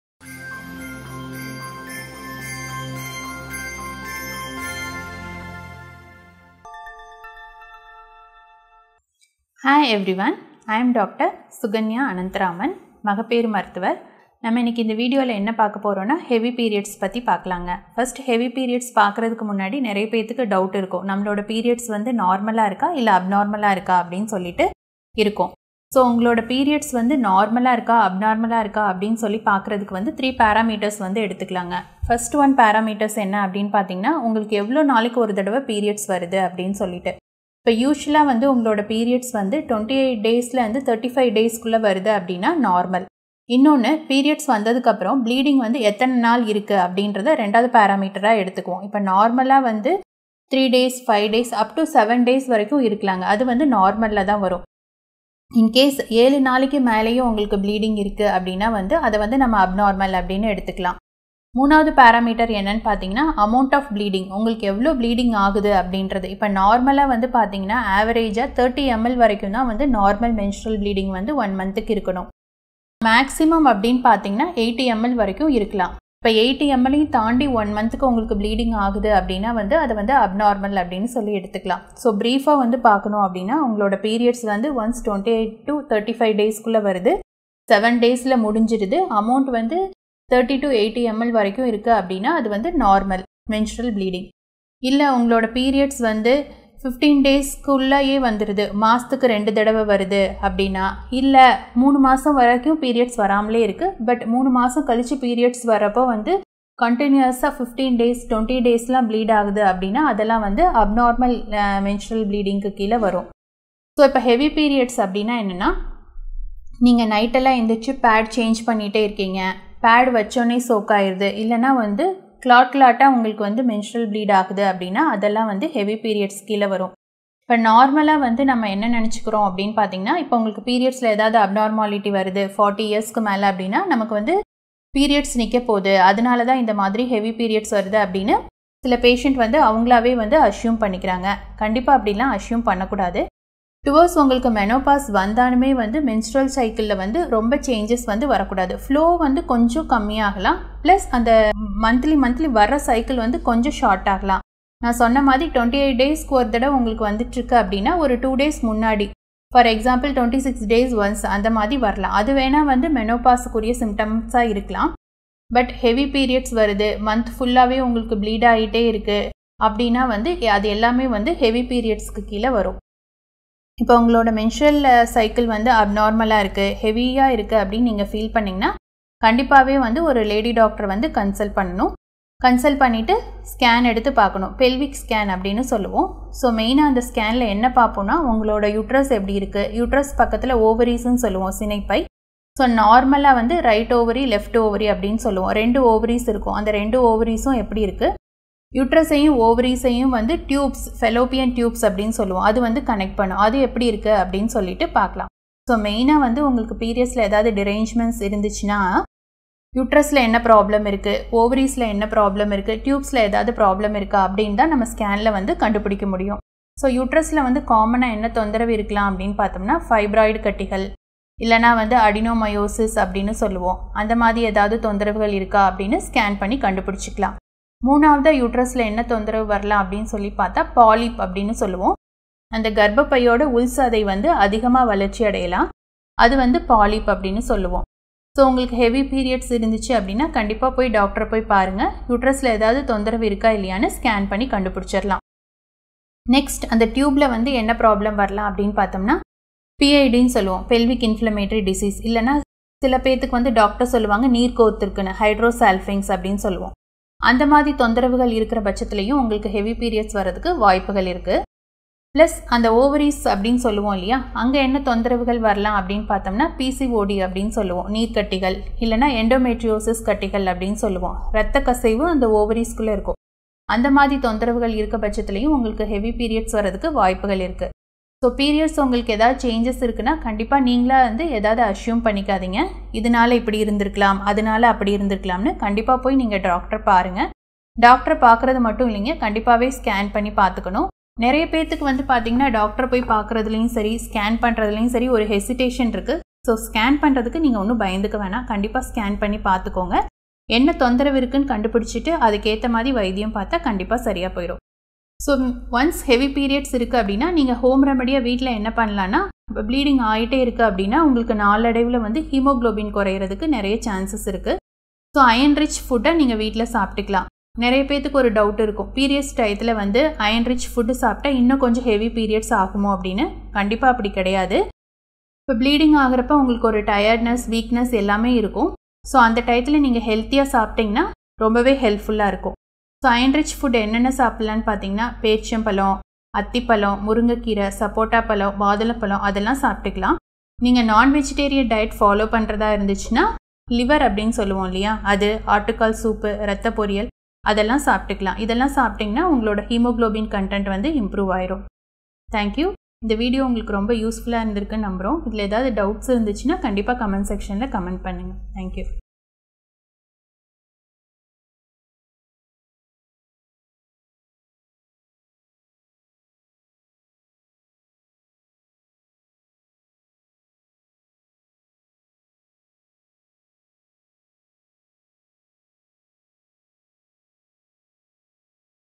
Heavy periods: அதிக மாதவிடாய் உதிரப்போக்கு | Causes for Heavy Periods | Menstrual Bleeding
அதிக மாதவிடாய் உதிரப்போக்கு
Causes for Heavy Periods
இந்த பதிவு உங்களுக்கு பிடித்துருந்தால் உங்கள் நண்பர்களுக்கு பகிரவும்

Chapters
00:00-Intro
00:27- Peroids normal or abnormal
00:31- First parameters
00:47- Second parameters
01:57- Heavy Periods
03:04- Why Scan for Heavy Periods
03:39- Common Uterus Problems
04:24- Common Problems in Ovary
05:15- Diet for Heavy periods
05:40- Iron-Rich Foods
05:46- Iron-Rich Non-Veg Foods

Clinic Address:
244, NSR Road, Saibaba Colony, Coimbatore